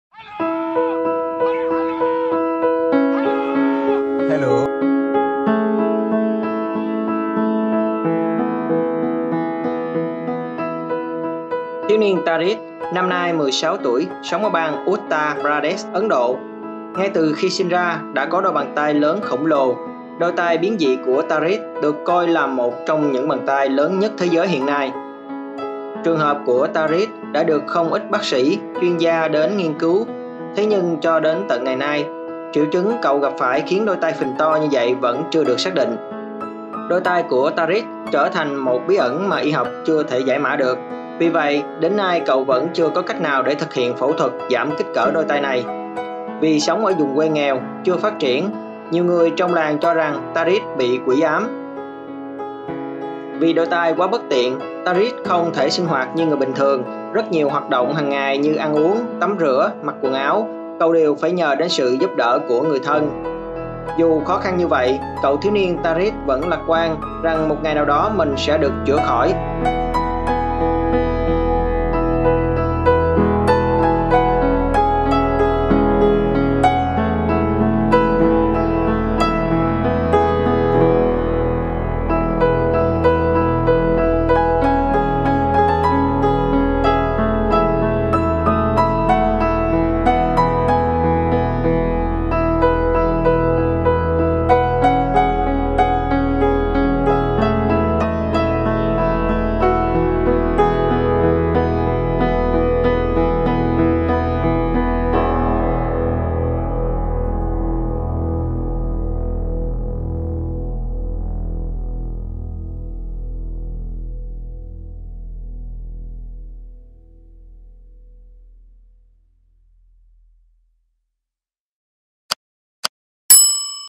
Thiếu niên có “đôi tay quỷ” khổng lồ | Báo Người Lao Động
Thiếu niên Tarik, năm nay 16 tuổi sống ở bang Uttar Pradesh, Ấn Độ ngay từ khi sinh ra đã có đôi bàn tay lớn khổng lồ. Đôi tay biến dị của Tarik được coi là một trong những bàn tay lớn nhất thế giới hiện tại.

* Thực hiện: Thanh Long